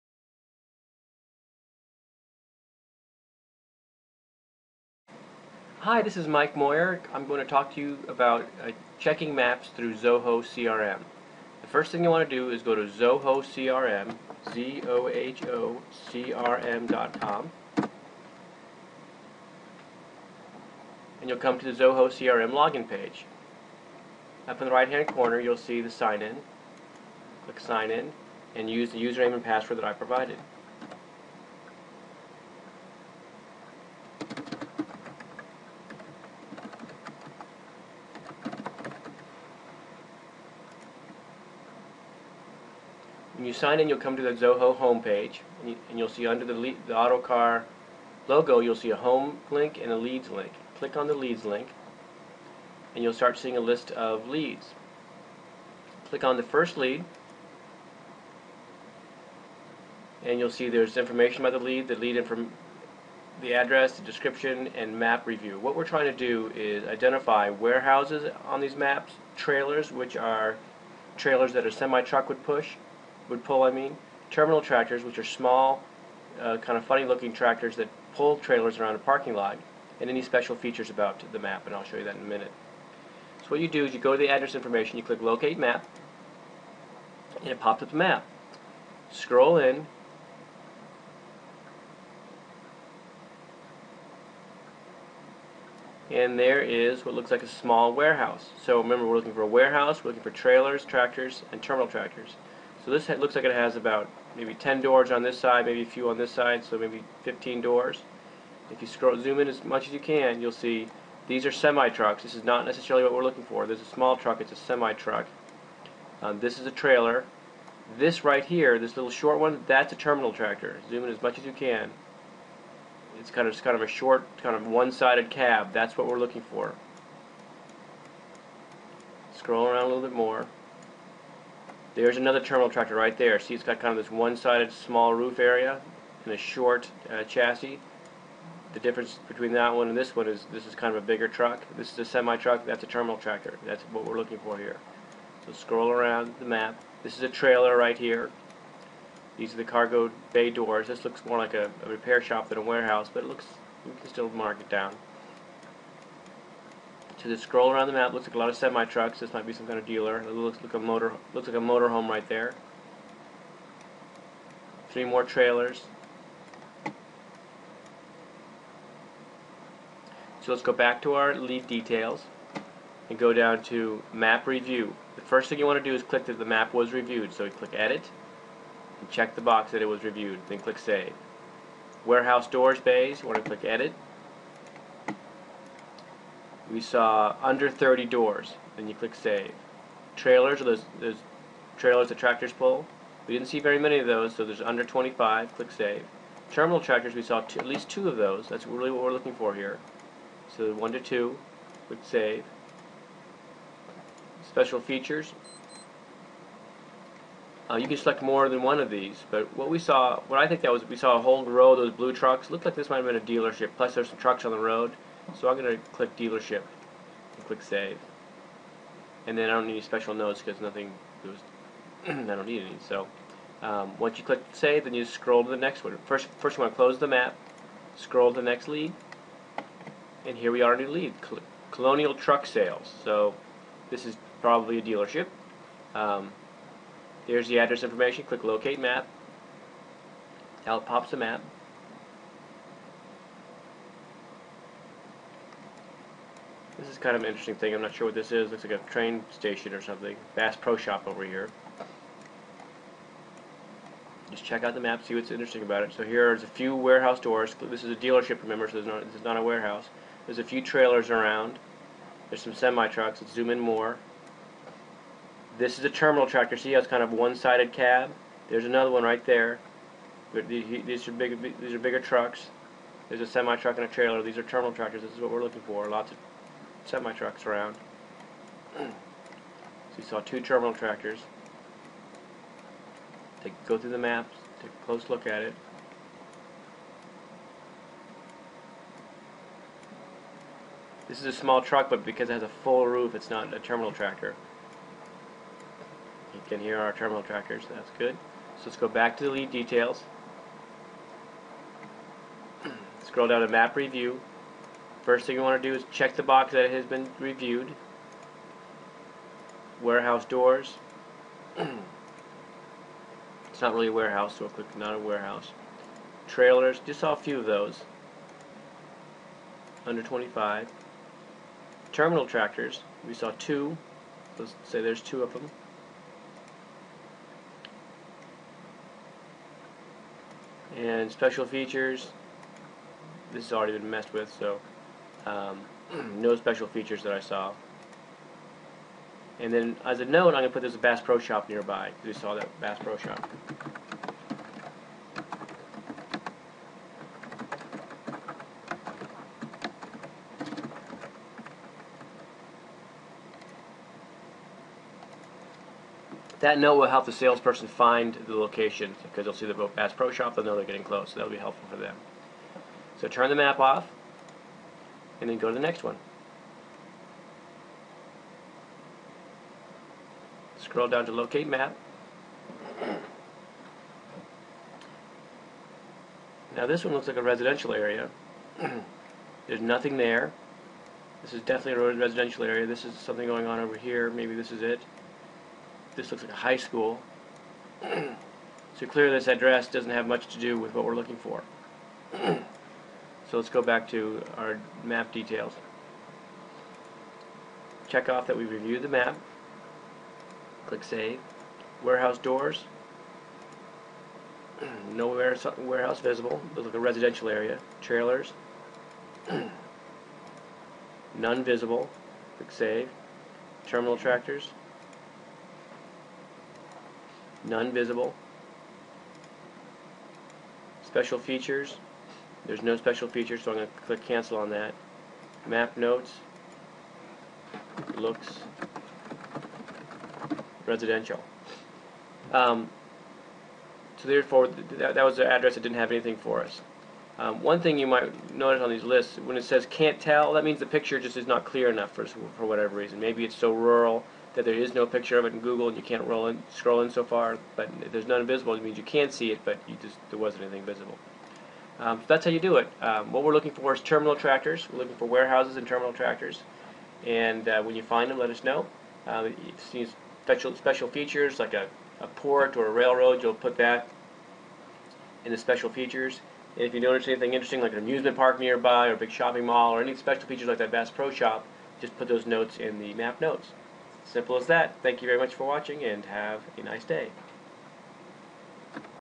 Reviewing Lead Maps in Zoho CRM
Here is how to review a map and update a lead in Zoho